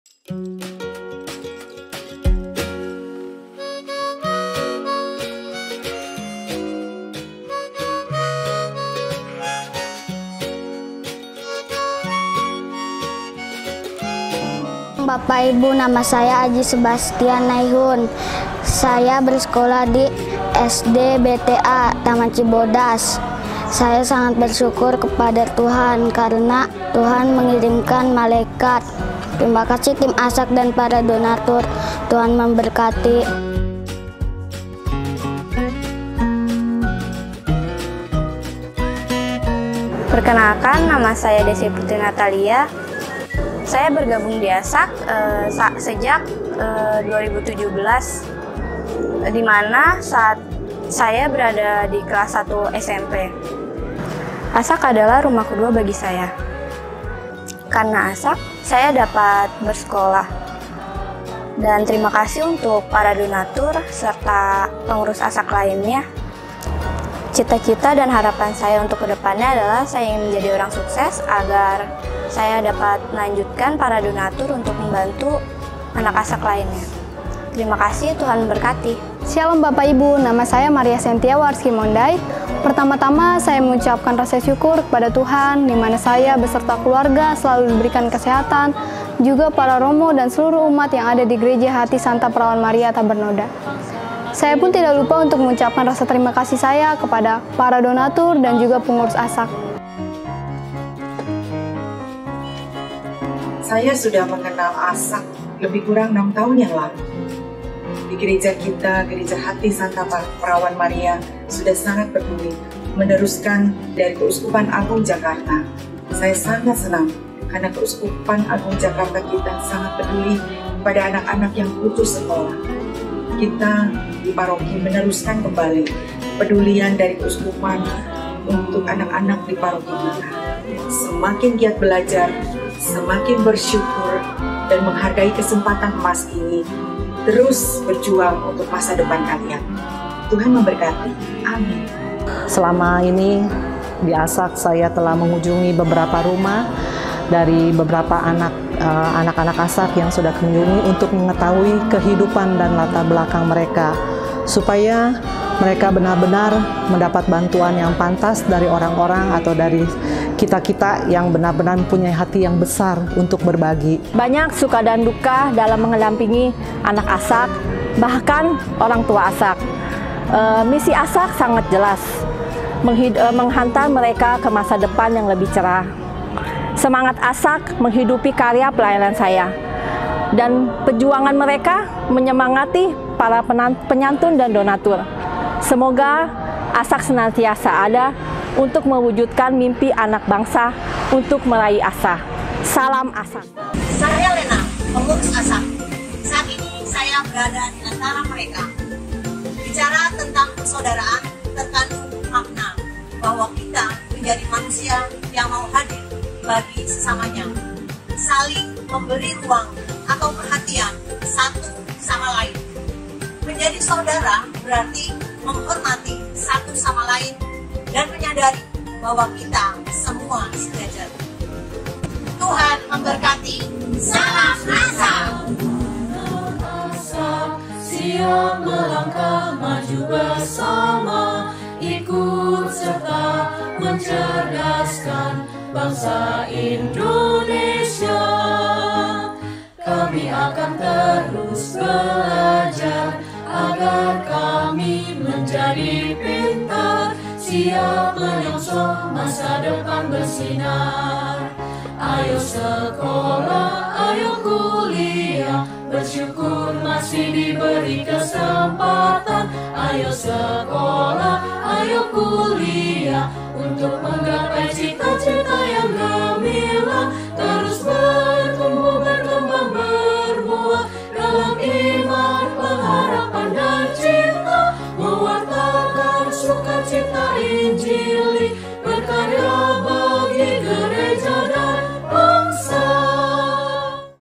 Hari Ulang Tahun ASAK - HSPMTB ke-11
Kesan dan pesan dari beberapa anak ASAK dari perwakilan SD hingga kuliah
Juga pesan yang disampaikan oleh pengurus ASAK dan sejumlah umat lingkungan paroki Tangerang.

Bantuan serta donasi untuk ASAK dapat disalurkan melalui rekening
BCA KCP Modern No. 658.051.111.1
an. PGDP Paroki Gereja Hati Santa Perawan MTB Tangerang